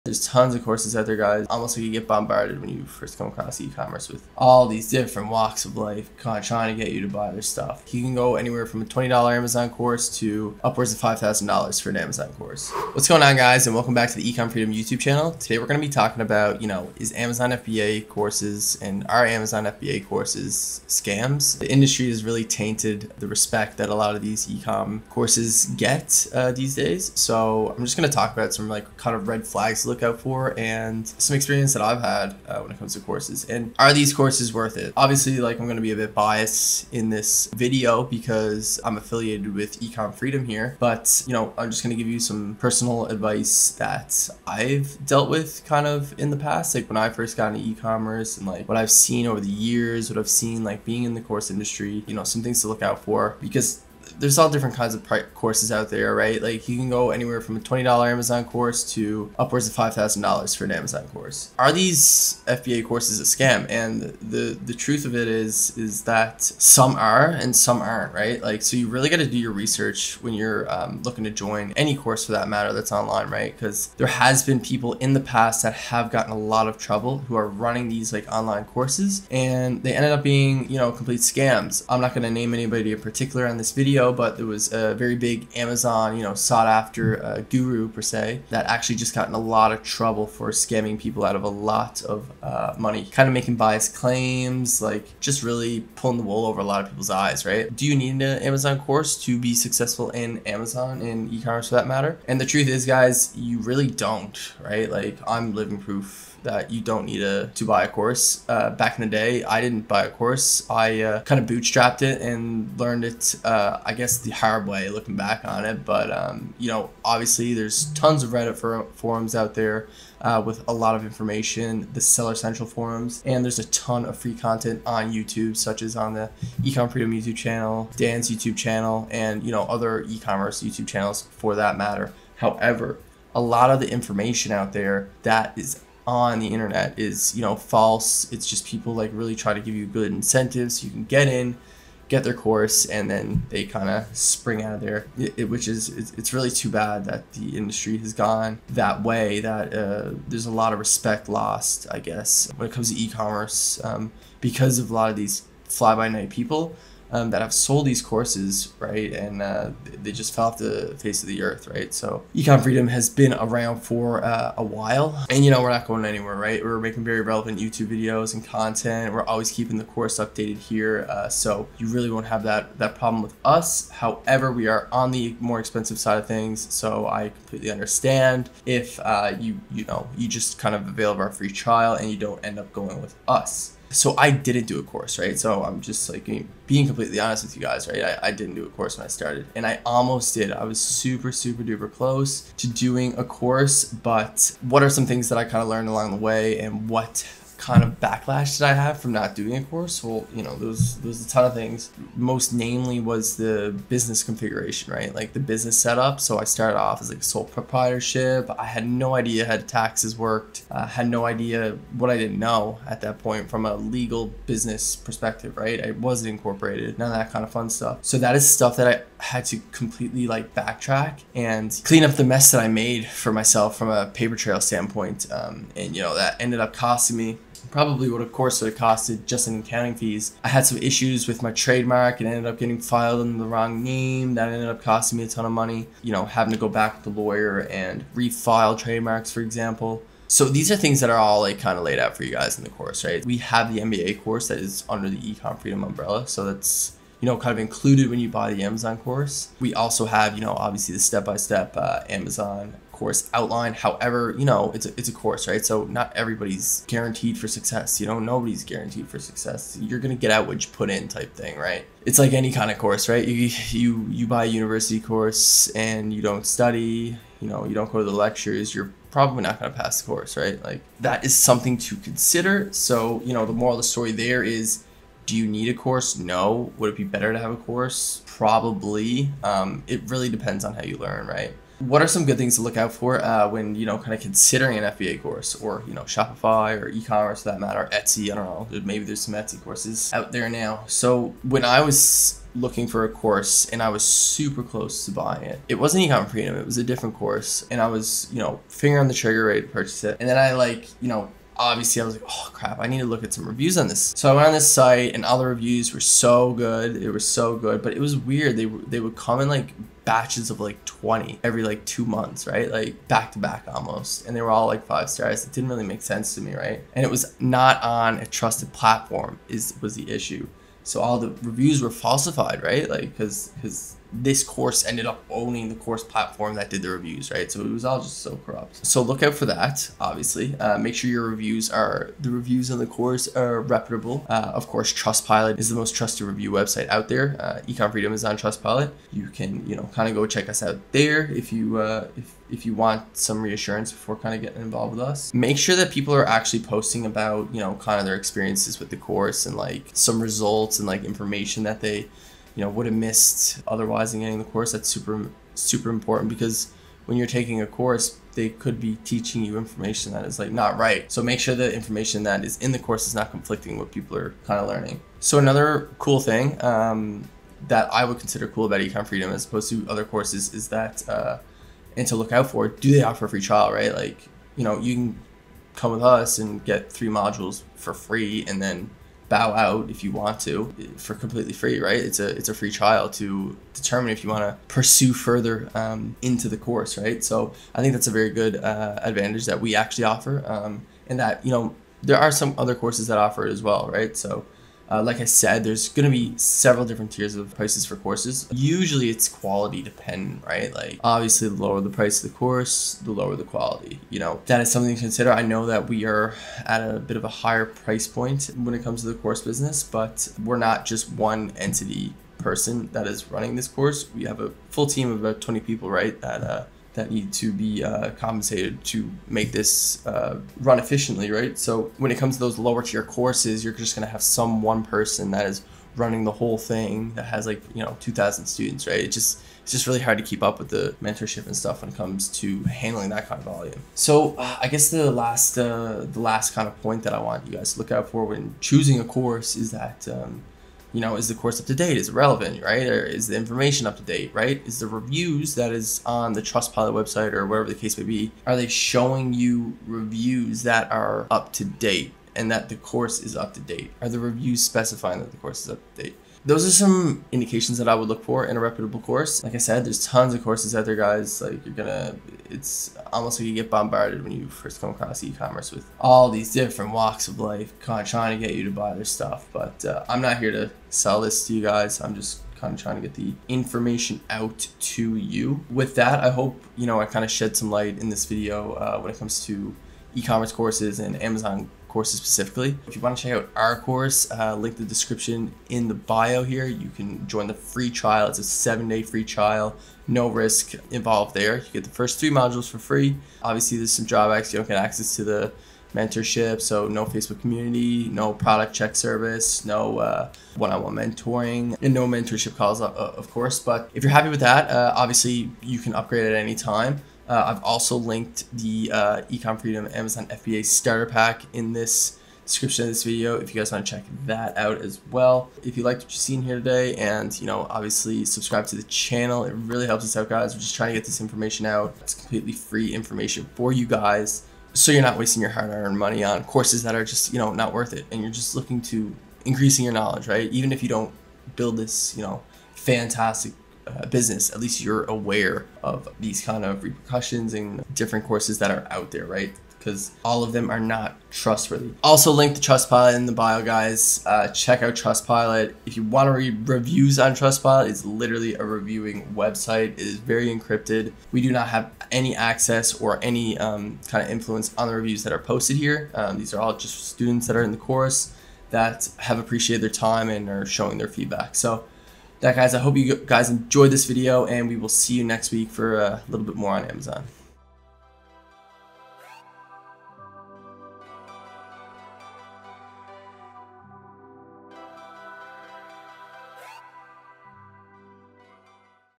Are Amazon FBA Courses a SCAM?
Welcome back to the Ecom Freedom channel!

Video Description:
In this video, our Head of Amazon mentor Jeff shares his thoughts on eCommerce courses, particularly Amazon FBA Courses, and what you should look out for if you decide to purchase them. He also indicates what these online courses should have so that you can make sure you aren't being scammed.

Jungle Scout: Best Deal Available Online (#1 Amazon Product Research Tool)

Get Valuable Tools To Start Your Shopify Business For Free

0:00 Introduction

1:37 Do you need to take an online Amazon FBA course to be successful?

3:14  What to expect when not taking an Amazon FBA course + my personal experiences

4:41 What to look out for when looking before purchasing an online Amazon FBA course?

7:55 Final thoughts